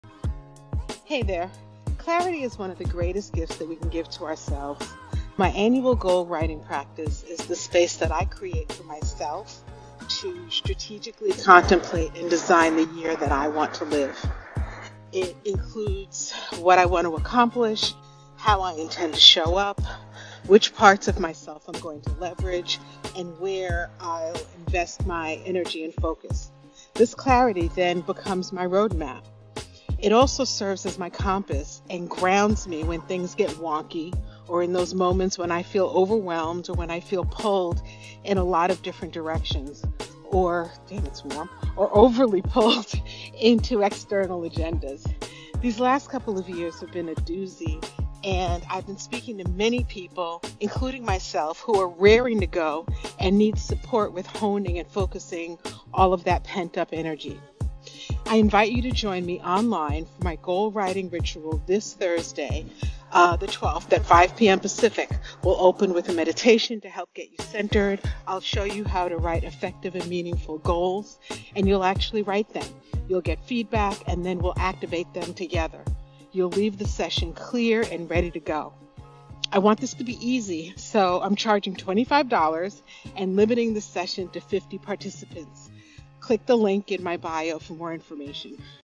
Plan Your Year Goal Writing Workshop
Join me for 90 minutes on Thursday, 1/12/23 and start your year setting yourself up for success. This is a hands-on and experiential workshop during which you will:
* Discover the pitfalls that often stand in the way of reaching new year’s goals,
* Learn how to create effective goals that matter,
* Create and activate meaningful goals and a roadmap for 2023.

The tools you will learn can be applied to any area of life where you’d like to see tangible progress, improvement, or a change.

I look forward to supporting you and I really hope you will join me!

~Lisane